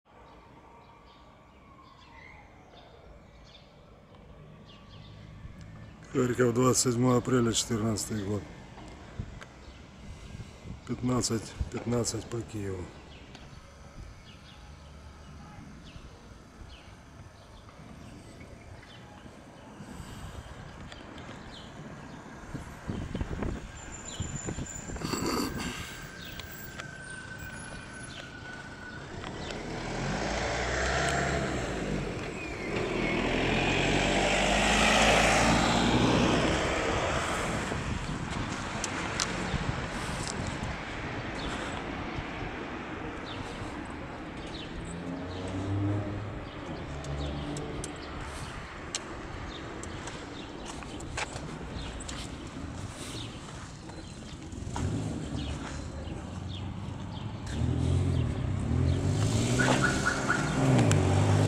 Кот - граффити на ул. Гражданская, 27 апреля 2014 , Харьков
Потрясающе натуральный рисунок кота на воротах жилого дома в Харькове по улице Гражданская.
27 апреля 2014, граффити.